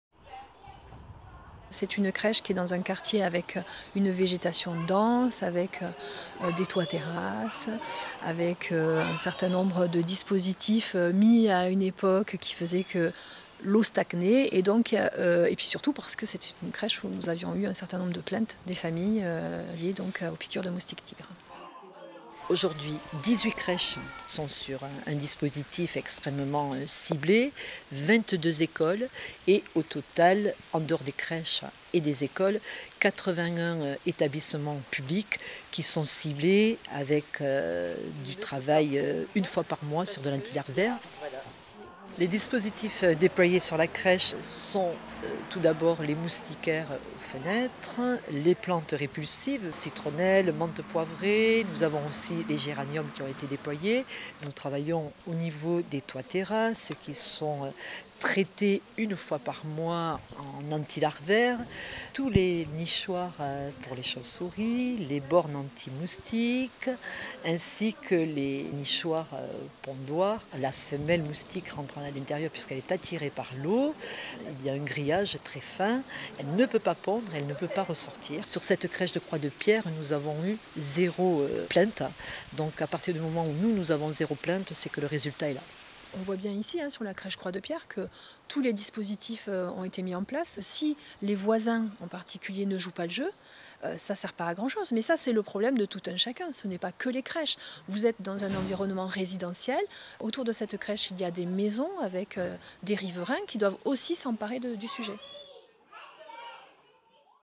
Stop aux moustiques-tigres
Comme plusieurs autres crèches et écoles toulousaines, la crèche Croix-de-Pierre a été équipée d’un ensemble de dispositifs visant à repousser les moustiques-tigres.